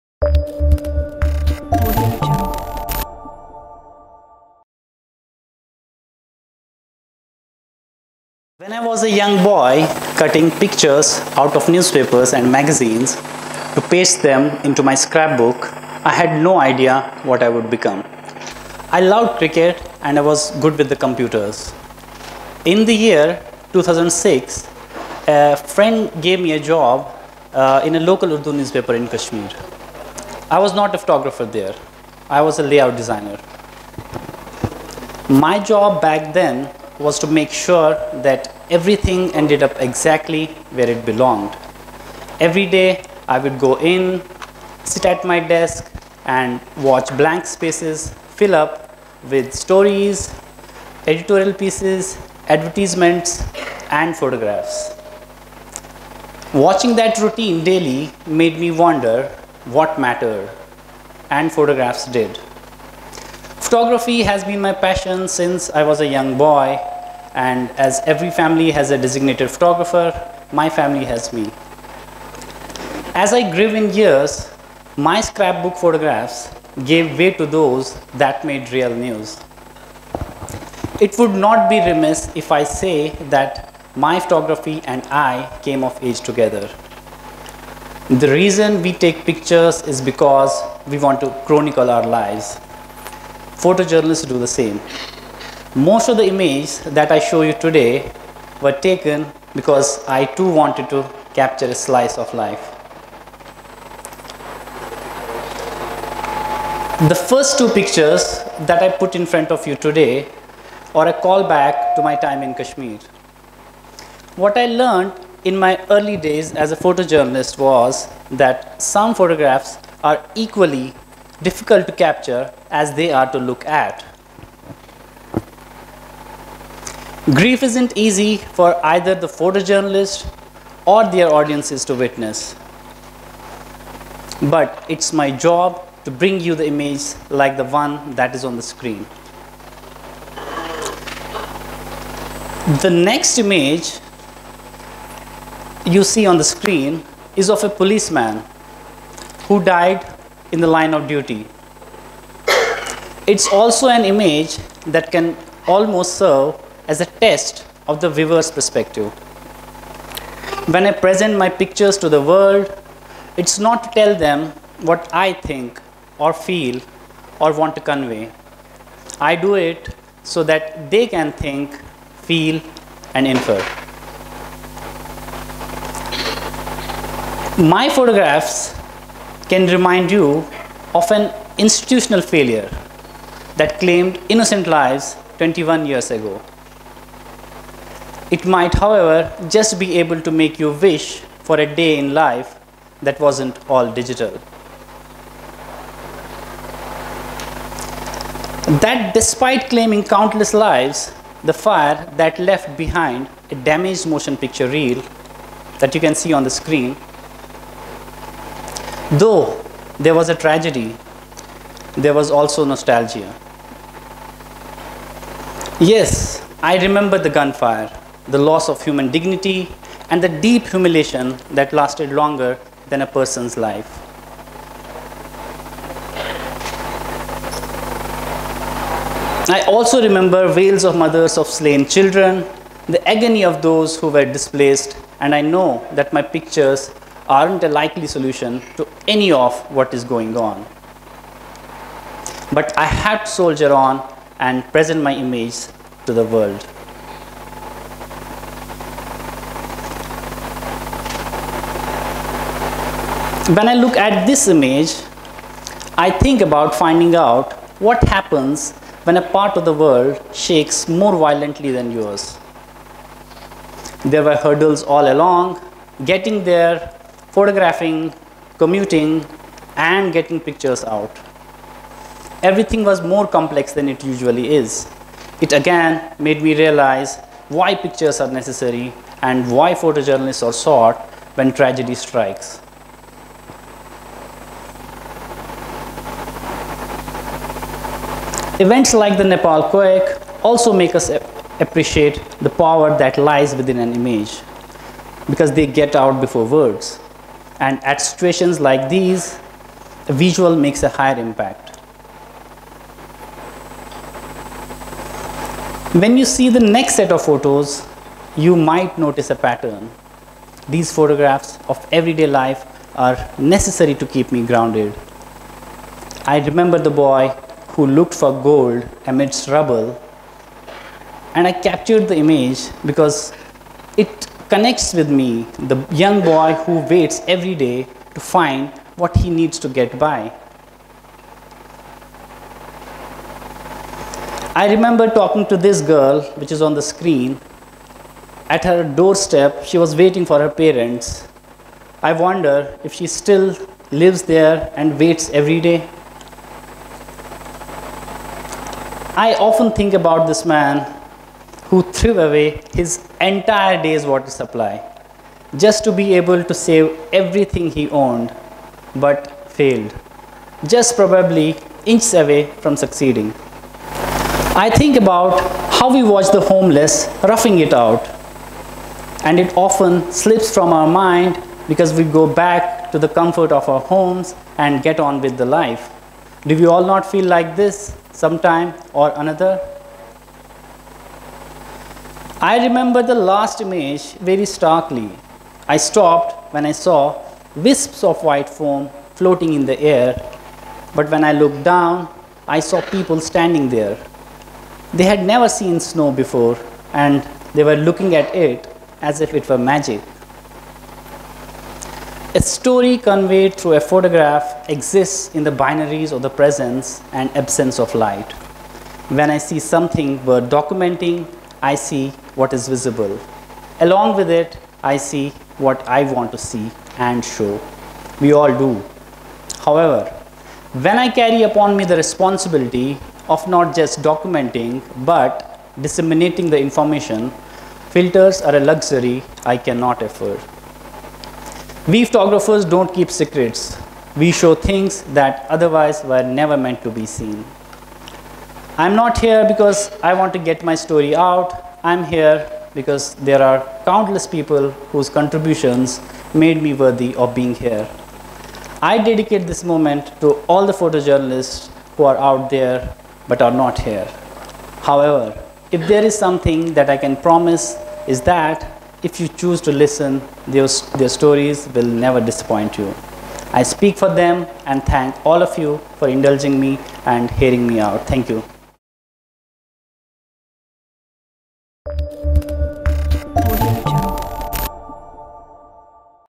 Story behind my lens | Burhaan Kinu | TEDxKCCEMSR
Burhaan is a photojournalist, right from the roots of most happening journalism in India - Kashmir. His work has been published in TIME magazine, Huffinginton post, CNBC and many more international publications. Here, he talks about his experience behind the lens.  Photojournalist This talk was given at a TEDx event using the TED conference format but independently organized by a local community.